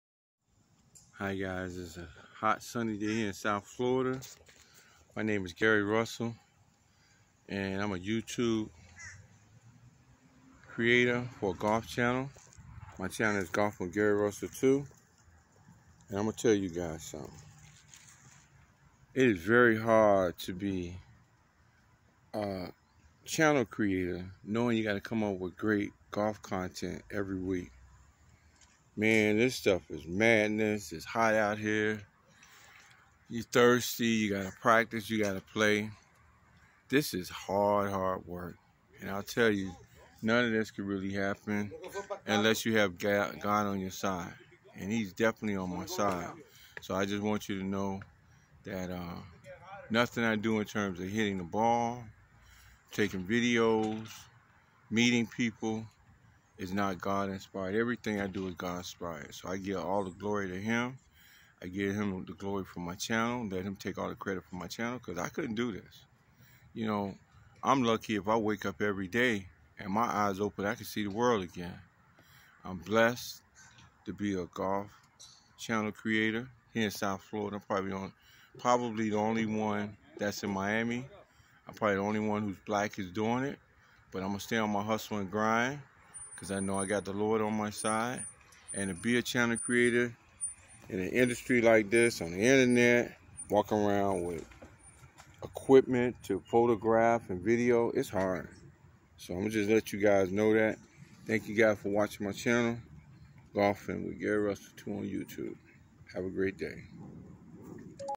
Great news 👏in SoFlo: This video is not sad, but one that gives all praise 🙌 glory to God.
Hey 👋guys,  it is truly a blessing to do what I do. When I decided to become a channel creator on YouTube. I didn't know it was hard 😕work.

However,  I give all thanks and praise to God for making my golf channel available to a world 🌎 audience.

I am proud to call myself a channel creator in golf where the number of  black professional golfers can be counted on hands.

I hope to change that narrative and bring black in champions in golf from SoFlo by creating more access and opportunities to our young at earlier ages.

I am 305 and MIA dude for life I grind for my city  and the inner city community. icedaddymia 🏌️‍♀️  💪💪